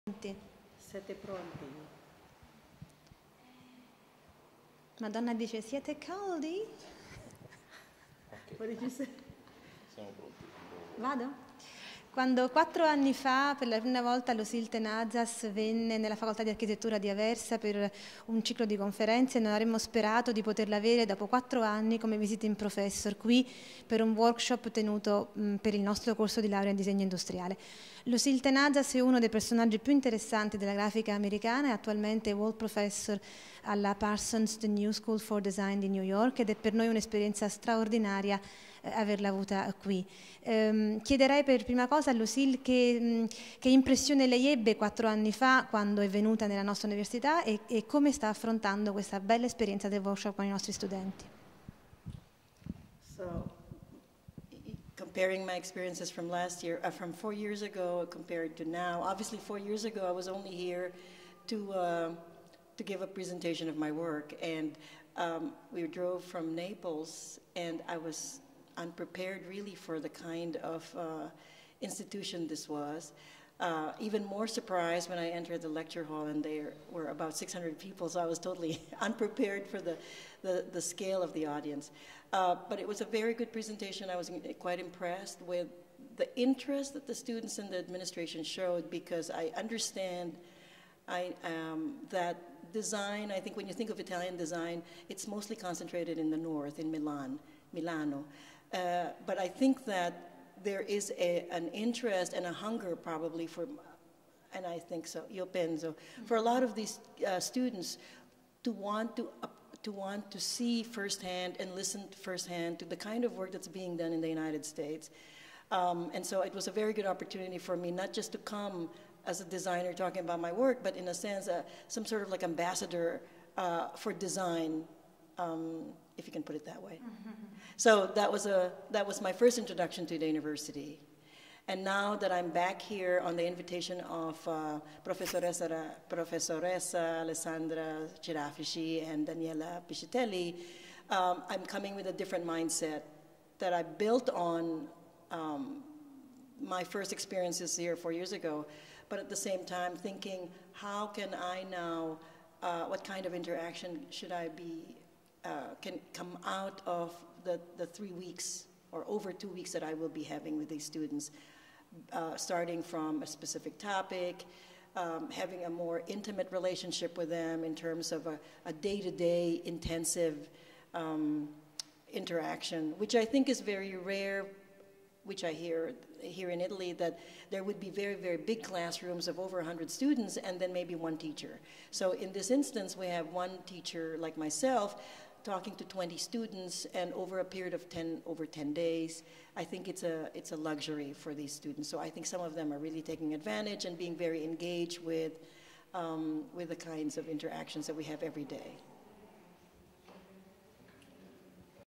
Lucille Tenazas
workshop di Lucille Tenazas presso il Dipartimento di Architettura e Disegno Industriale Luigi Vanvitelli